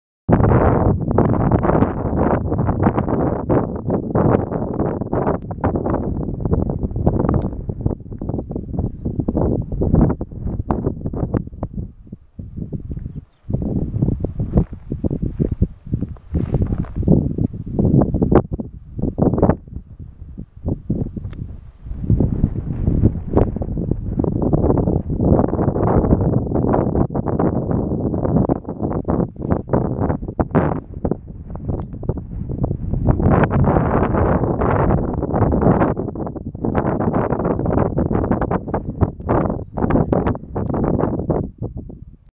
ESP - CAN - Sierra del Hornijo - 360º desde Pico San Vicente (913 mtrs)
Panorámica de 360 grados tomada el día 25/04/2021 desde la cima de la Pico San Vicente, por la Sierra del Hornijo, en Cantabria, en España.